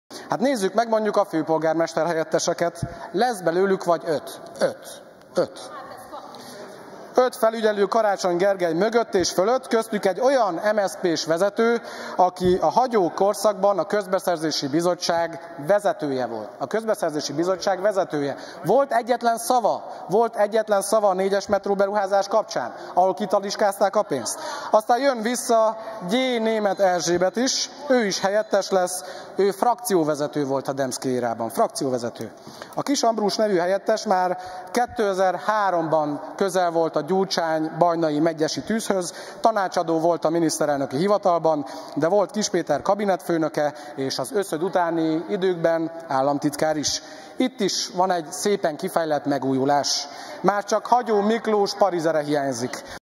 Öt helyettese is lehet Karácsony Gergelynek
Öt, azaz öt helyettese is lehet Karácsony Gergelynek. Egyenesen a múltból érkeznek, már csak Hagyó Miklós parizere hiányzik.